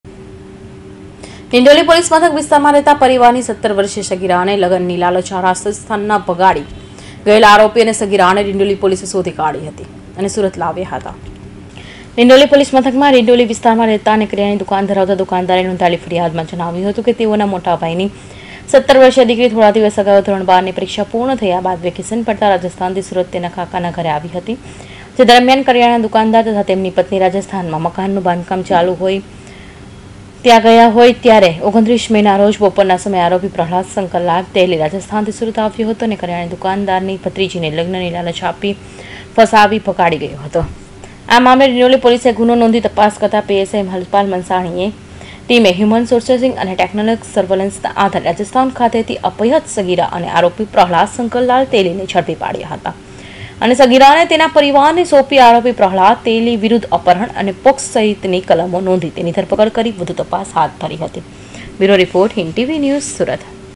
ડીડોલી પોલીસે સગિરાને ભગાડી જનાર આરોપીને ઝડપી લીધો
લગ્નની લાલચે 17 વર્ષિય સગિરાને ભગાડી જનાર આરોપી ઝડપાયો